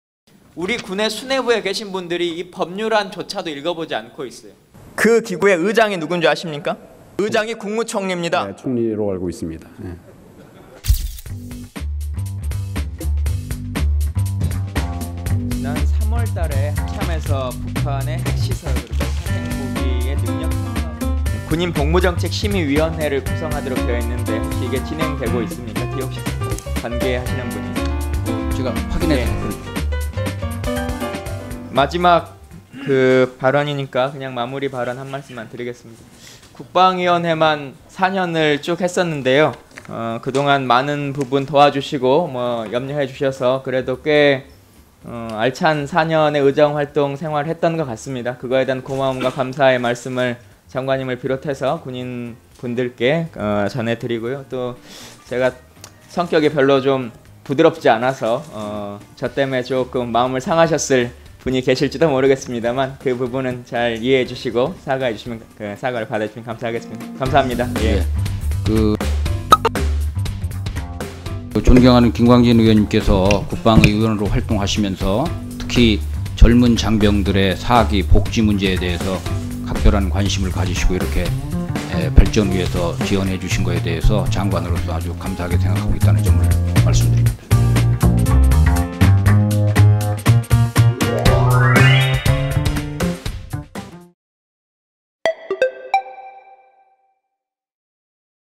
[VIDEOMUG] 김광진 임기 마지막 회의… 한민구의 아름다운 작별 인사 / SBS
5월 3일, 국회에서 열린 국방위원회 전체회의에서 한민구 국방부 장관이 더불어민주당 김광진 의원에게 아름다운 작별인사를 건네는 장면이 포착됐습니다.

두 남자의 아름다운 작별인사를 SBS 비디오머그에서 전해드립니다.